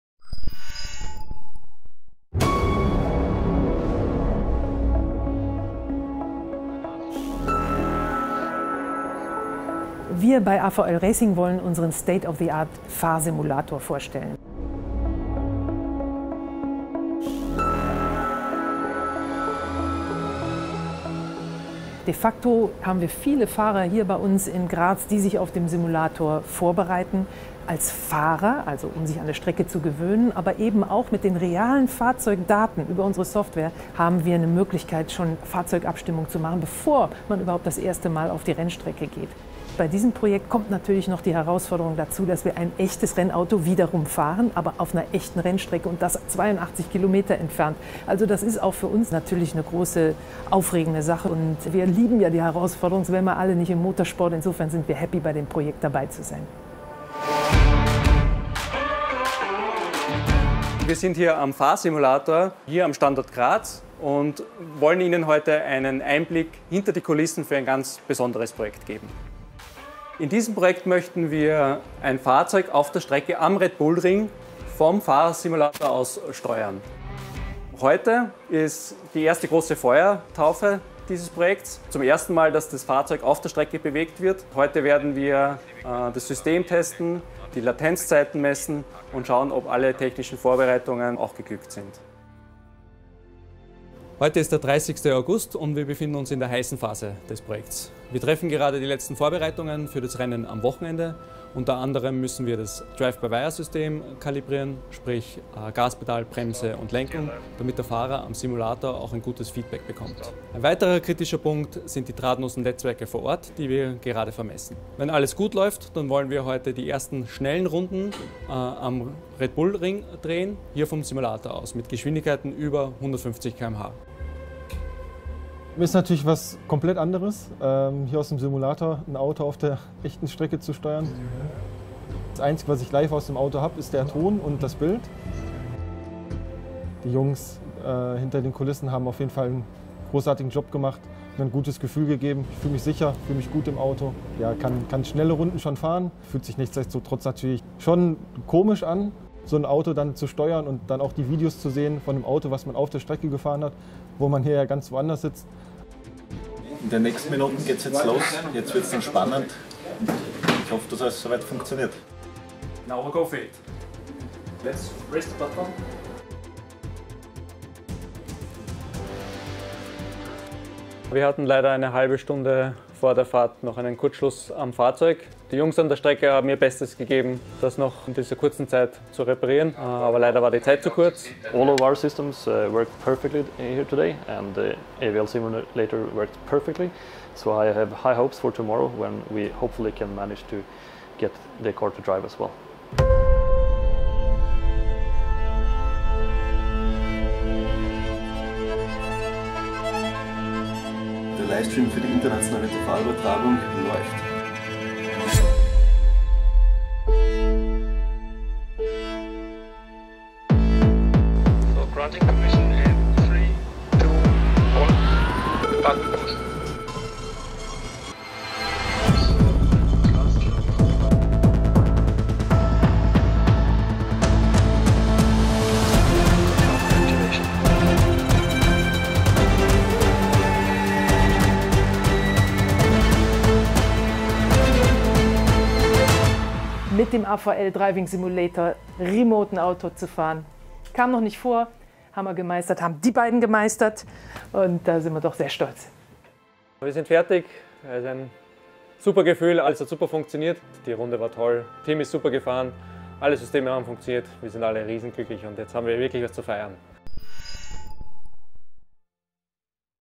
(DE) Hinter den Kulissen: Fahrerlos mit 150km/h über den Red Bull Ring - DTM Electric Remote Run
Stellen Sie sich vor, Sie steuern einen Rennwagen in voller Fahrt über eine Rennstrecke, während Sie fast hundert Kilometer entfernt in einem Simulator sitzen. Diese Vision ist Wirklichkeit geworden.

Tim Heinemann (DTM Trophy Champion 2020) nahm im Hightech-Fahrsimulator von AVL in Graz Platz, um das DTM Electric Demo Car aus 82 Kilometern Entfernung um den Red Bull Ring zu steuern.

Ohne selbst am Steuer oder gar im Cockpit zu sitzen, konnte der 23-jährige Rennfahrer das Auto steuern, indem er Feedback zu Parametern wie Handling und Stabilität des Fahrzeugs erhielt.